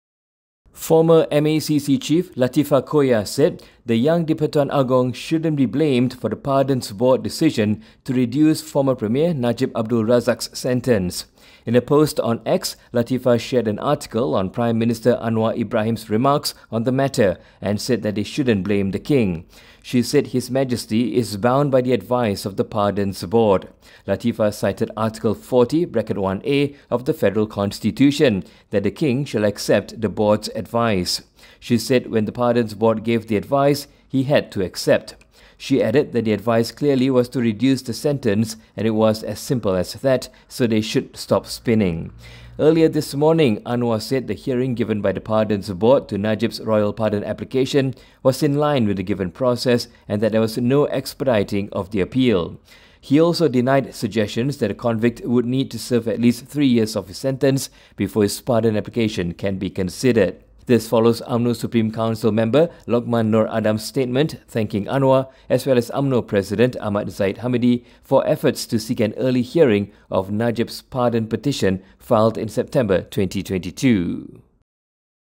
Don't blame Agong for Pardons Board decision, Latheefa tells Anwar
Former MACC chief Latheefa Koya has hit out at Prime Minister Anwar Ibrahim’s latest remark defending a Pardons Board decision to reduce Najib Abdul Razak’s sentence.

Latheefa accused Anwar of shifting the blame to the Yang di-Pertuan Agong as chairperson of the Pardons Board.

Latheefa said His Majesty is bound by the advice of the Pardons Board.